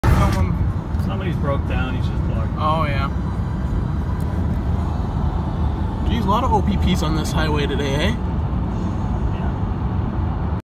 OPP Dodge Charger On a traffic stop
Enjoy the video of this police car 🚓 conducting a traffic stop on 401 eastbound.